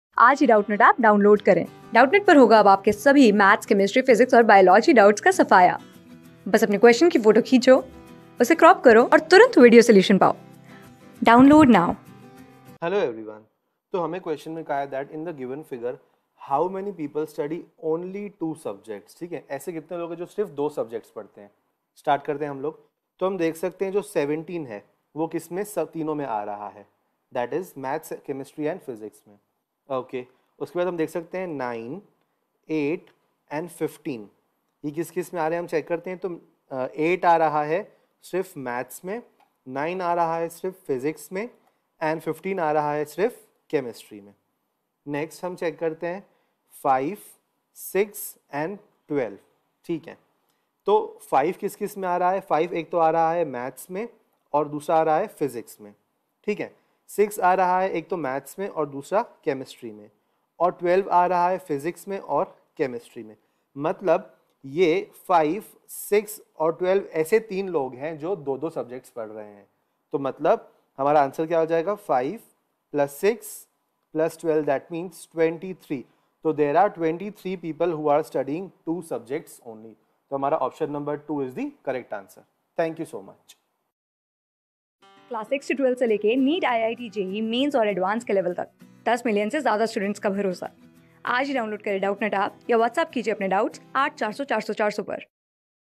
In the given figure, how many people study only 2 subjects? \n\n\n | CLASS 14 | Practice Set 10 ...
In the given figure, how many people study only 2 subjects? \n\n\n
Class: 14
Subject: MATHS
Chapter: Practice Set 10
Board:GOVT

👉 Have Any Query? Ask Us.